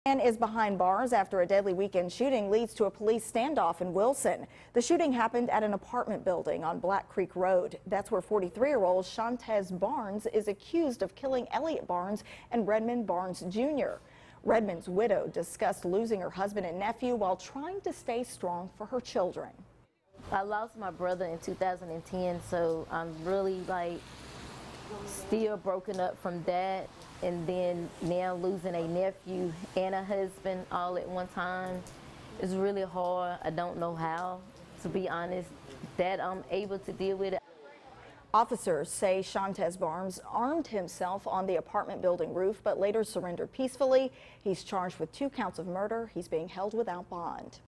2 dead in Wilson shooting, suspect arrested after rooftop standoff
2 dead in Wilson shooting, suspect arrested after rooftop standoff